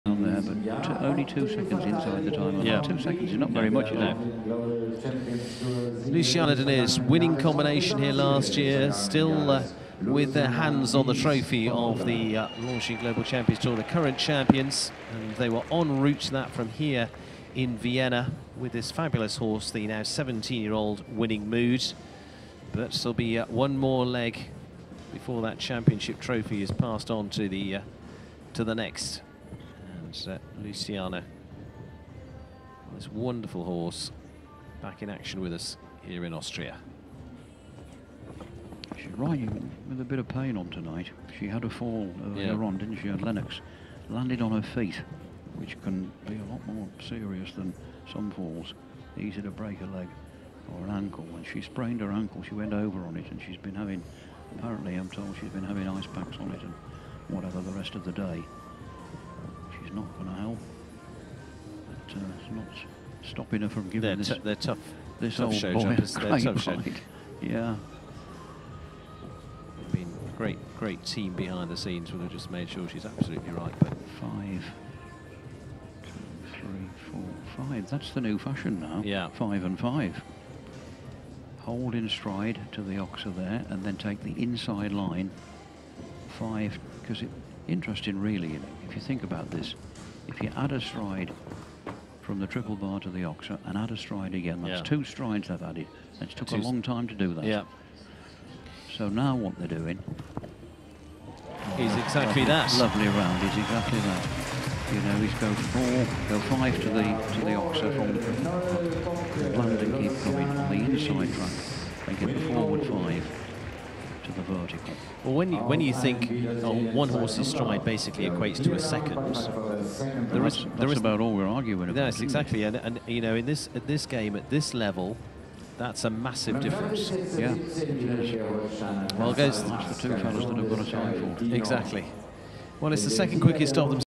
Luciana Diniz & Winningmood (2016) Vienna Global Champions Tour (1ª Round)
Vienna Masters 2016, Longines Global Champions Tour 2016;
Luciana Diniz with Winningmood they reached the 5th place in the Grand Prix !!!